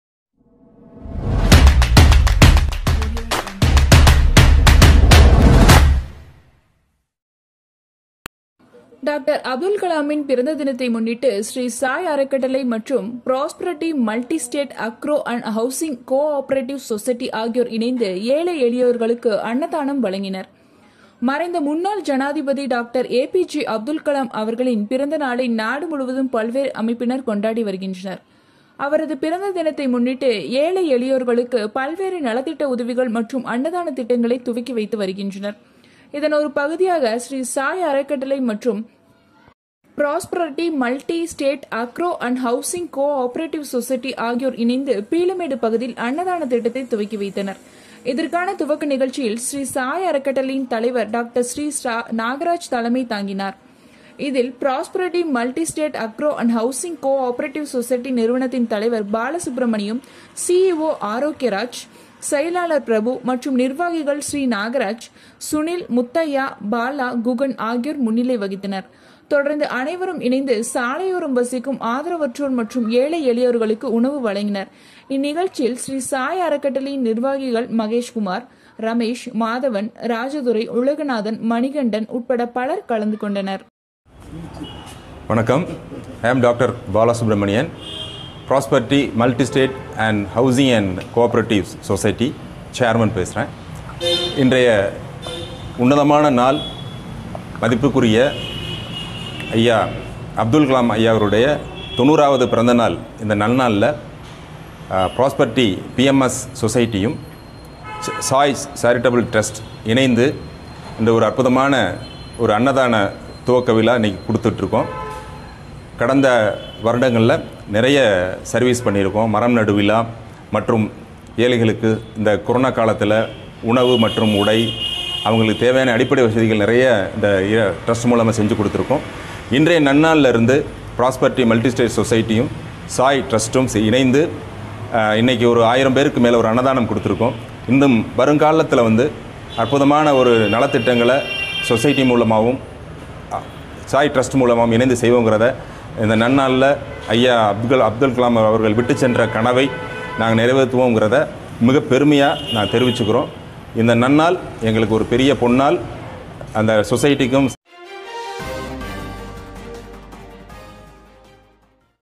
Annathanam on the occasion of Dr. ABJ Abdul Kalam's birthday
டாக்டர் அப்துல் கலாமின் பிறந்த தினத்தை முன்னிட்டு  ஸ்ரீ சாய் அறக்கட்டளை மற்றும் ப்ராஸ்பரிட்டி மல்டி ஸ்டேட் அக்ரோ அண்ட் ஹவுசிங் கோ ஆப்ரேட்டிவ் சொசைட்டி ஆகியோர் இணைந்து ஏழை எளியோருக்கு அன்னதானத்தை வழங்கினர்.மறைந்த முன்னாள் ஜனாதிபதி டாக்டர் ஏபிஜே அப்துல் கலாம் அவர்களின் பிறந்த நாளை நாடு முழுவதும் பல்வேறு அமைப்பினர் கொண்டாடி வருகின்றனர்.அவரது பிறந்த தினத்தை முன்னிட்டு ஏழை எளியோருக்கு பல்வேறு நலத்திட்ட உதவிகள் மற்றும் அன்னதான திட்டங்களை துவக்கி வருகின்றனர்..இதன் ஒரு பகுதியாக  ஸ்ரீ சாய் அறக்கட்டளை மற்றும் ப்ராஸ்பரிட்டி மல்டி ஸ்டேட் அக்ரோ அண்ட் ஹவுசிங் கோ ஆப்ரேட்டிவ் சொசைட்டி ஆகியோர் இணைந்து பீளமேடு பகுதியில் அன்னதான திட்டத்தை துவக்கினர்.இதற்கான துவக்க நிகழ்ச்சியில் ஸ்ரீ சாய் அறக்கட்டளையின் தலைவர் டாக்டர் ஸ்ரீ நாகராஜ் தலைமை தாங்கினார்.இதில், ப்ராஸ்பரிட்டி மல்டி ஸ்டேட் அக்ரோ அண்ட் ஹவுசிங் கோ ஆப்ரேட்டிவ் சொசைட்டி நிறுவனத்தின் தலைவர் பாலசுப்ரமணியம்,சி.இ.ஓ.ஆரோக்கியராஜ்,செயலாளர் பிரபு,மற்றும் நிர்வாகிகள் ஸ்ரீ நாகராஜ்,சுனில்,முத்தையா,பாலா,குகன் ஆகியோர் முன்னிலை வகித்தனர்.தொடர்ந்து அனைவரும் இணைந்து சாலையோரம் வசிக்கும் ஆதரவற்றோர் மற்றும் ஏழை எளியோருக்கு உணவு வழங்கினர்.நிகழ்ச்சியில் ஸ்ரீ சாய் அறக்கட்டளையின் நிர்வாகிகள் மகேஷ் குமார்,ரமேஷ்,மாதவன்,ராஜதுரை,உலகநாதன்,மணிகண்டன் உட்பட பலர் கலந்து கொண்டனர்.